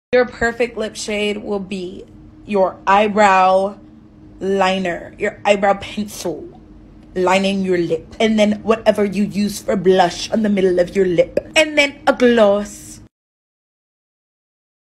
Perfect Lip Shade Hack
heckorhack ? Perfect Lip Shade Hack 👄

MY WORDS/EXPERIENCE :
Blush have less pigment as compared to lipsticks.
So, My lip pigmentation was peeking through so I used 2 layers of blush.
With this hack, you will get a natural looking lip color as compare to a lipstick coverage.
Its a hack not heck 😂 and it will look good with no makeup - makeup looks n minimal makeups.